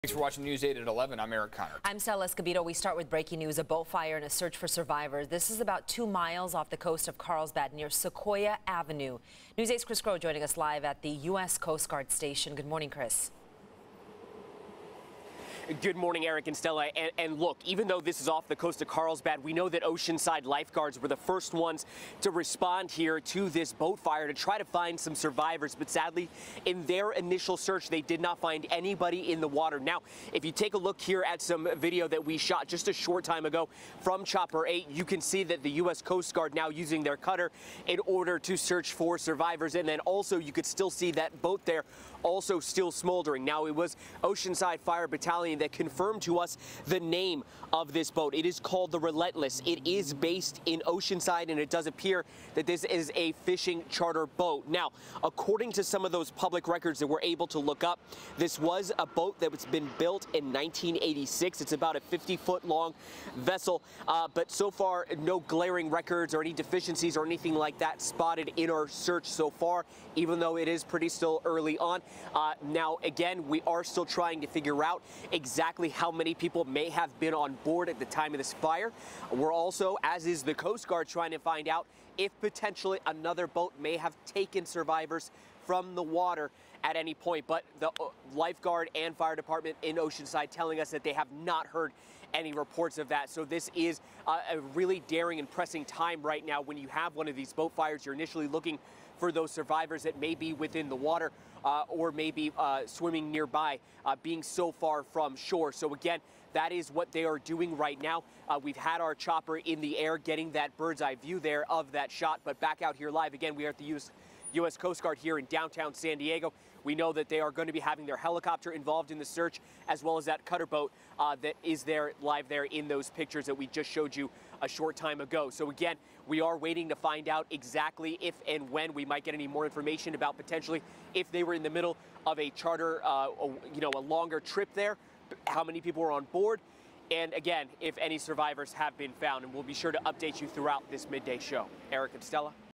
Oceanside fishing vessel catches fire off Carlsbad coast; Coast Guard searching for survivors
Oceanside Fire Battalion Chief confirmed with News 8 the boat that caught fire is the Relentless, a fishing boat based out of Oceanside.